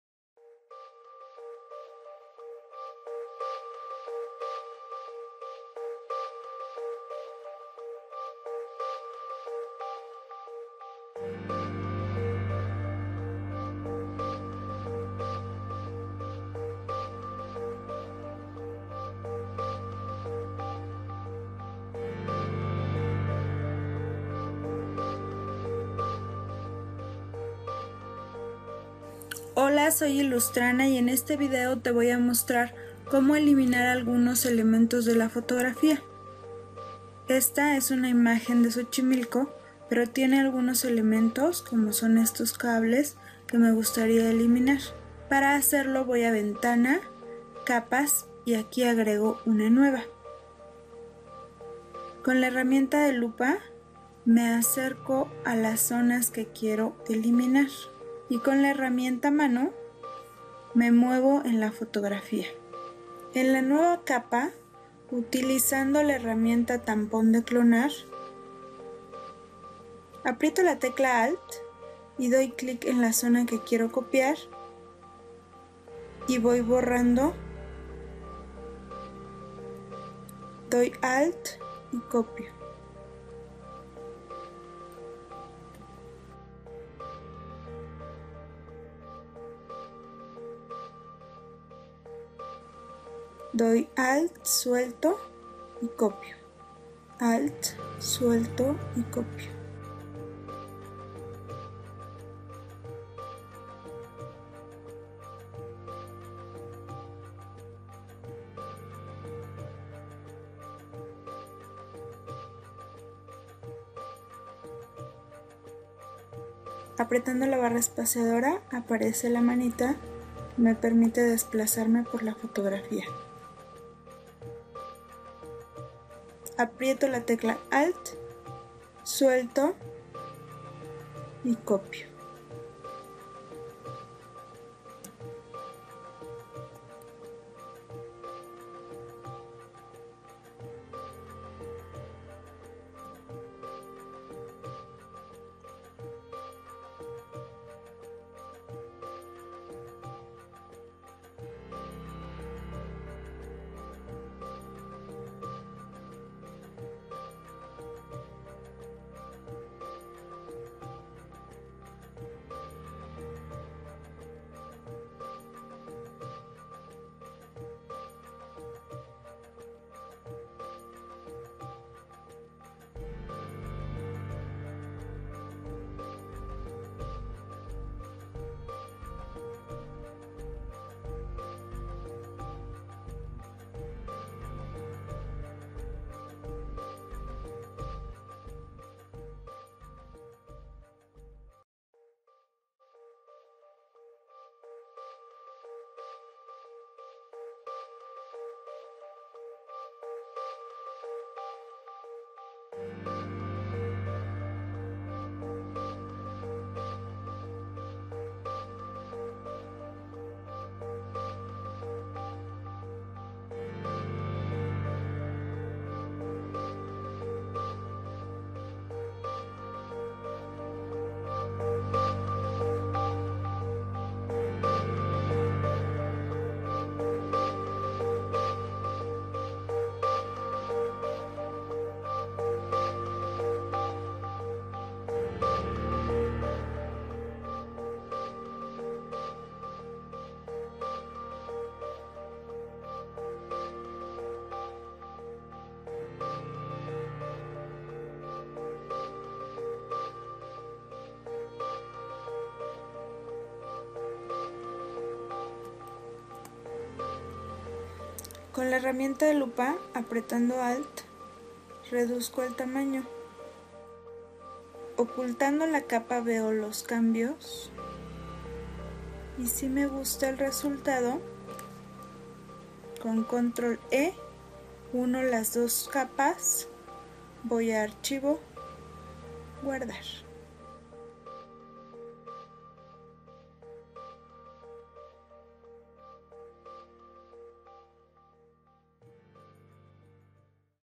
Cómo Eliminar Elementos de una foto
Ninguna foto o escenario es perfecto y por eso en este tutorial de Ilustrana aprenderás cómo limpiar tus fotos con capas y la herramienta lupa en Photoshop. Wacom30Tips30Días @Ilustrana

* Las sesiones con Ilustrana están enfocadas para las personas que están empezando su camino a potenciar su creatividad.